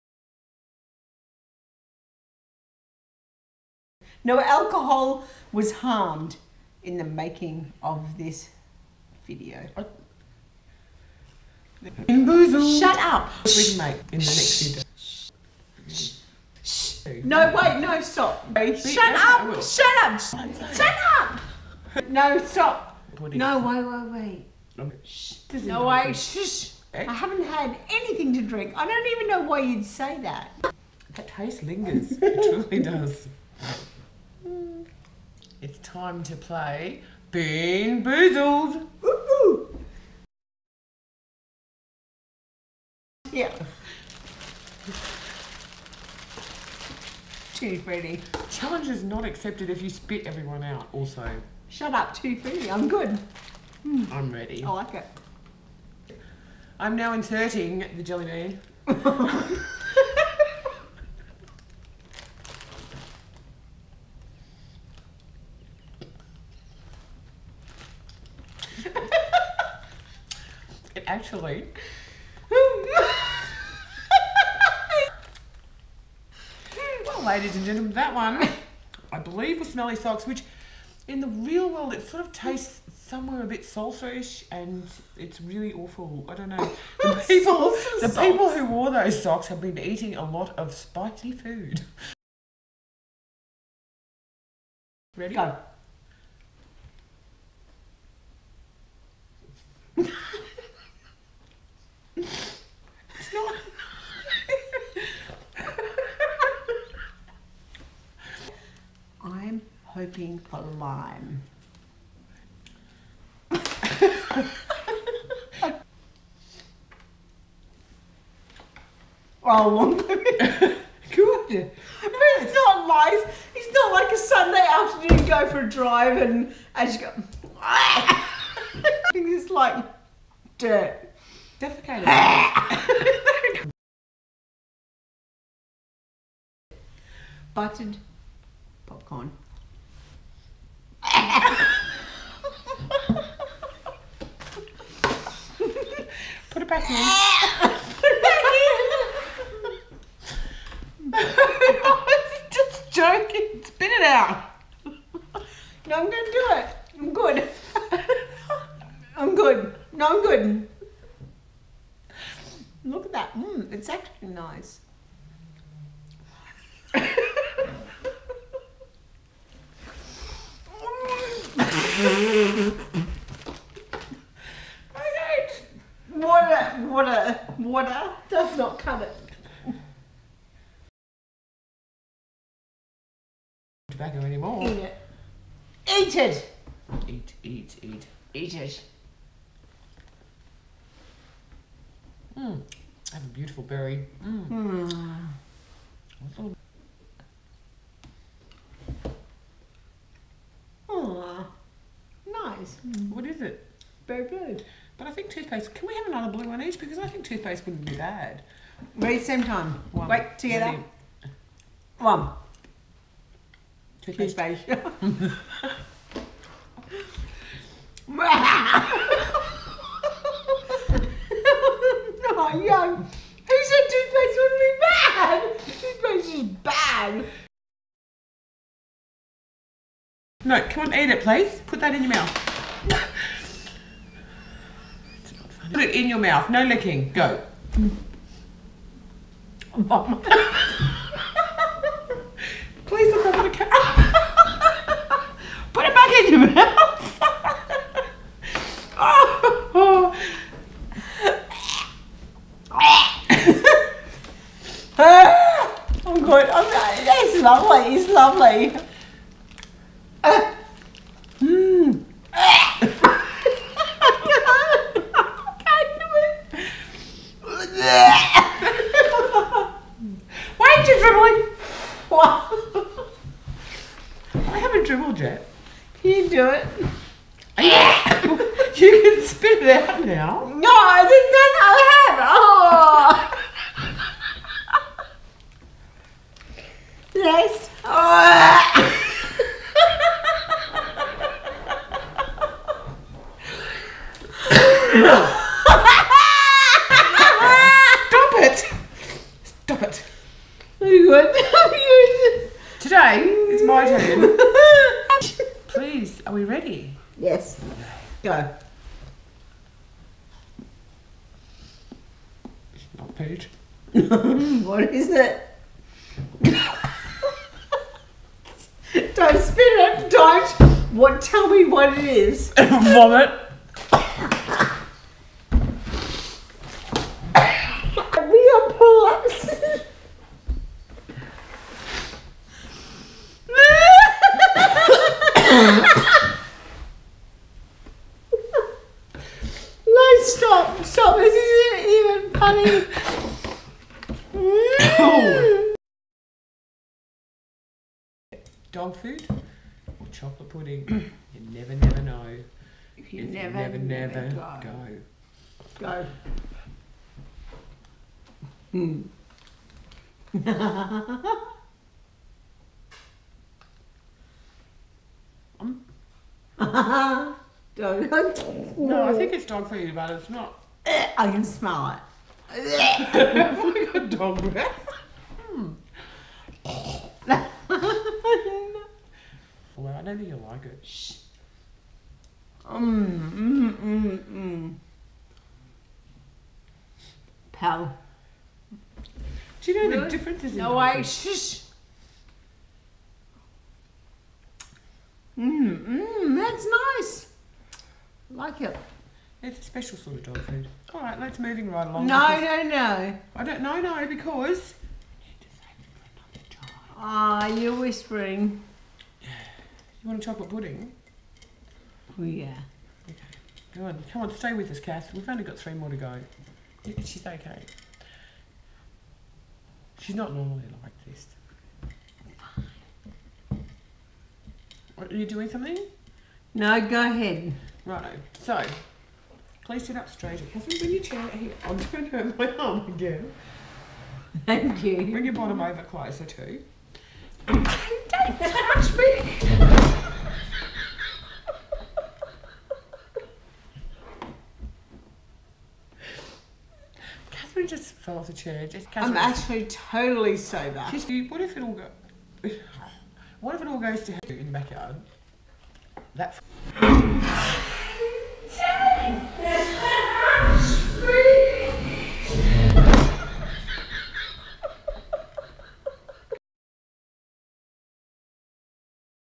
Bean Boozled with Wine
Two sisters doing the Bean Boozled challenge... with a drop or two of alcohol. See if you can work out which sister drank the wine! Finally a hilarious version of the popular Bean Boozled, that will give you a goody hearty laugh! If you can't watch the entire clip, skip to the last minute or two to see our side-splitting finale! Definitely worth the watch!